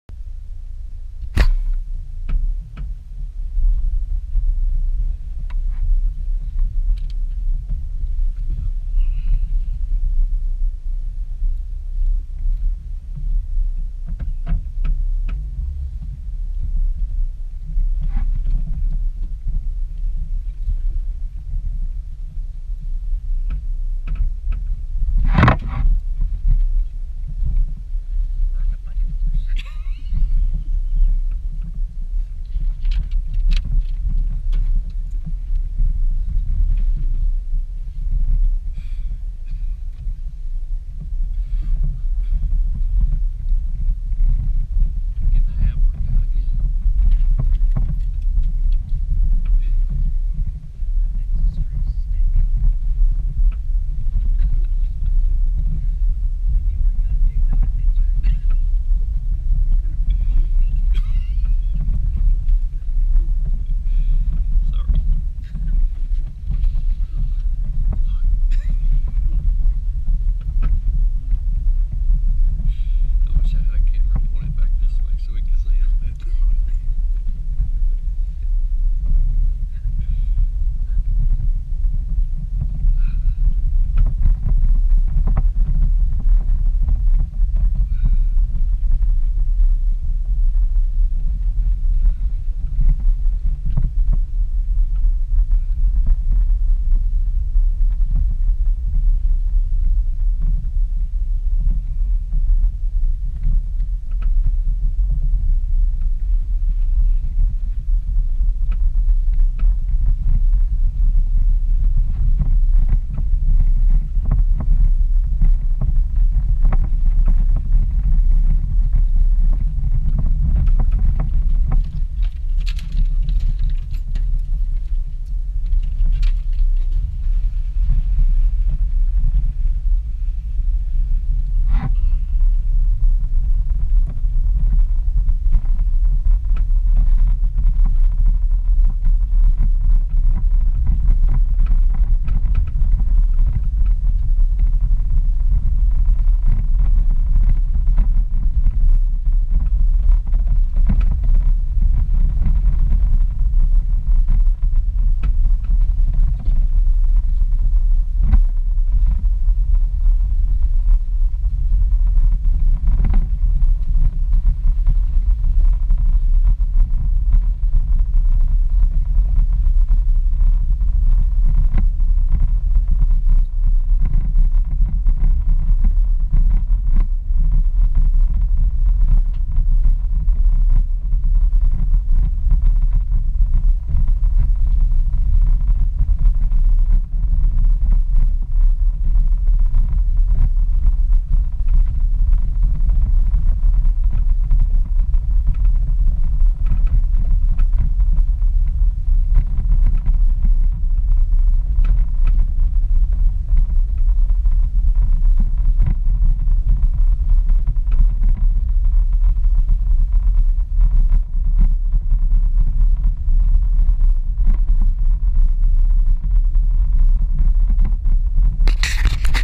Long Mountain
Little Side Trail that went up the side of Long Mountain.  Ended up at a Deer Stand.  Rough Rocky road that the wife said was like a "bucking horse" lol